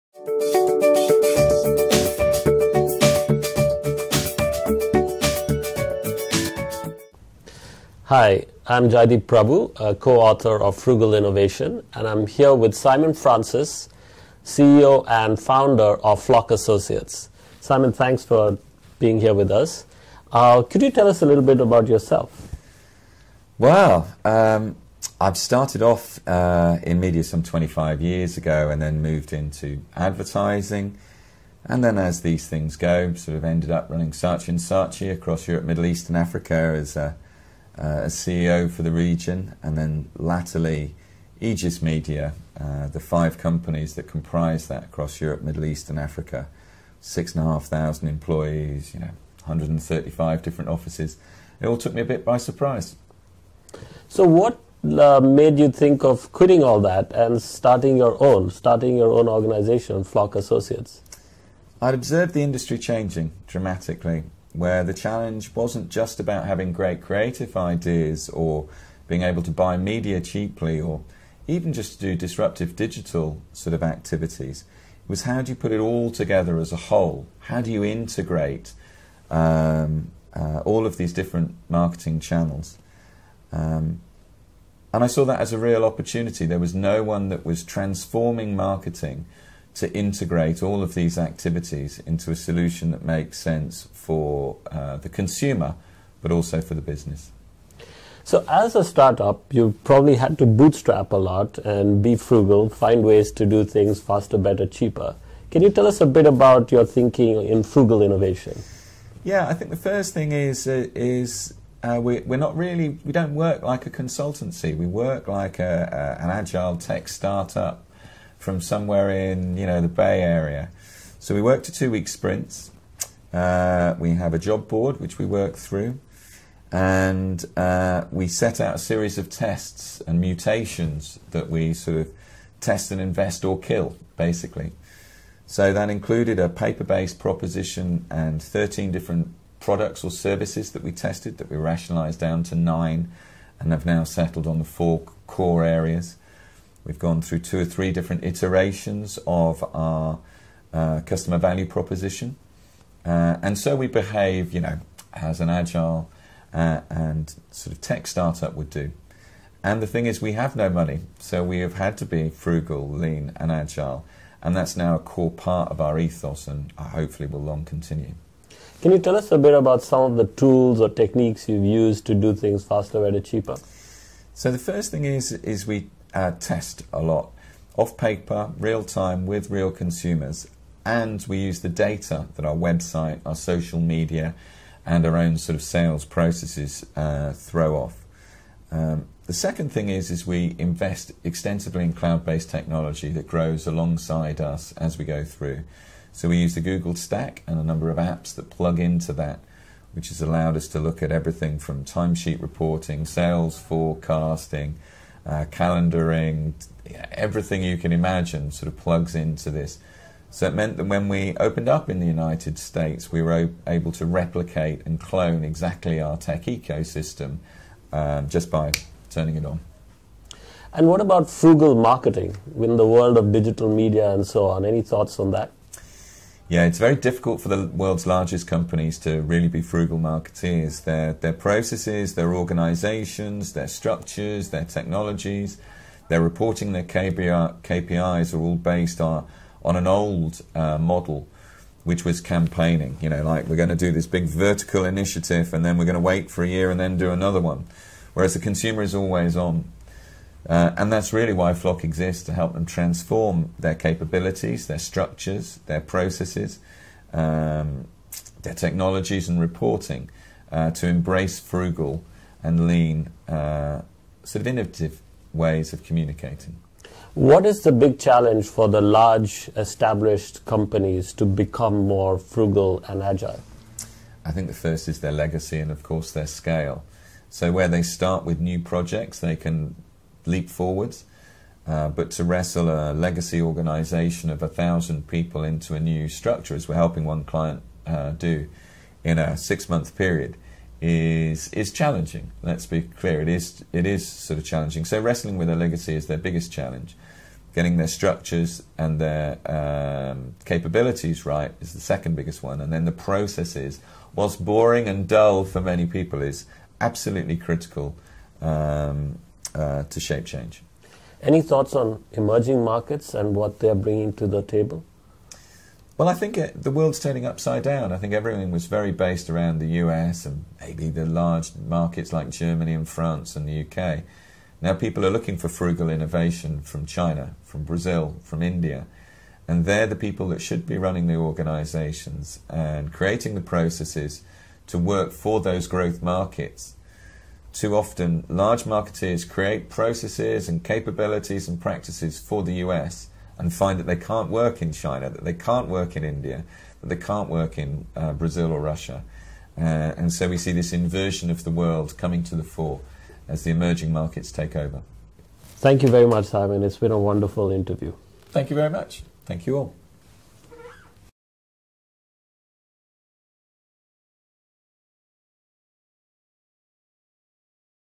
Leading Frugal Innovation: Simon Francis, CEO, Flock Associates
Simon Francis, CEO of Flock Associates explains what are the challenges of doing frugal marketing faster, better, and cheaper.
Flock is founded by world class global marketing leaders who design independent and objective marketing transformation solutions.